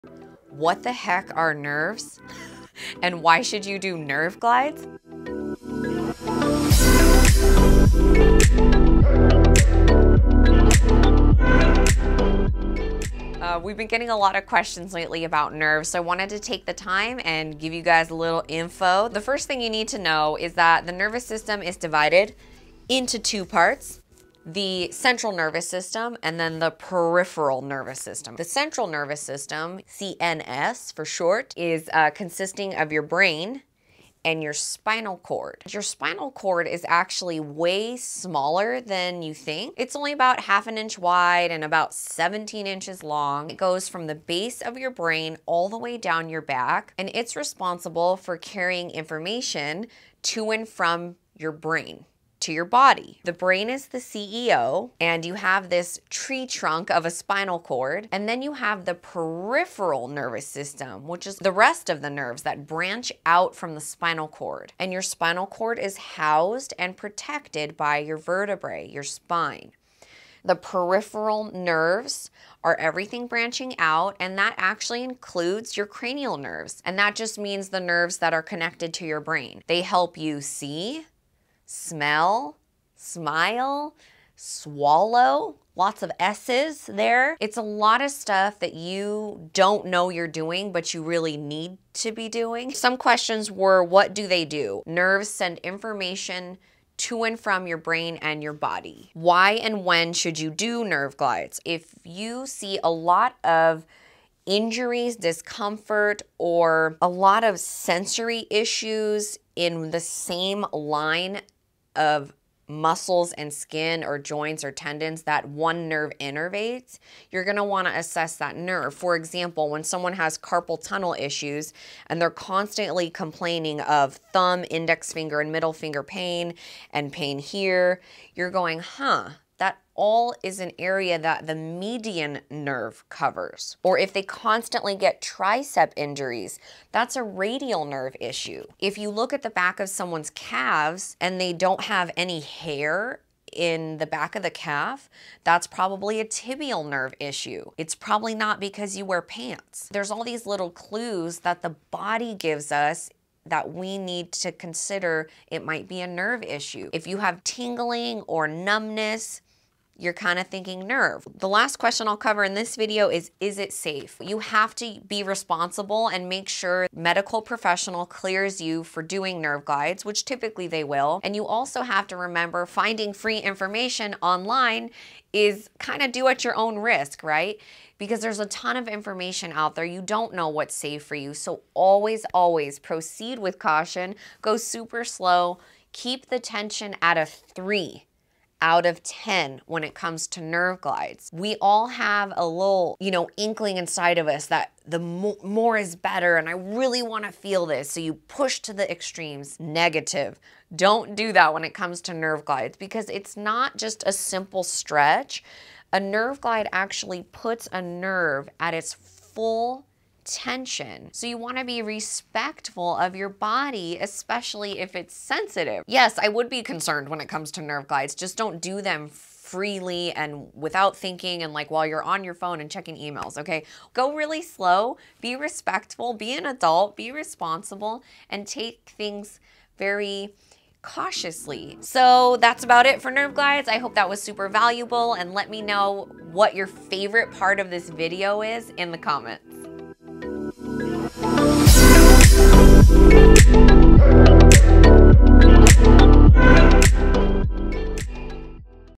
What Are Nerves and Nerve Glides?
We get a lot of questions surrounding Nerves and Nerve Glides so we thought this little explanation will help clarify what we're talking about and some cases we might use Nerve Glides to decrease pain and improve overall performance.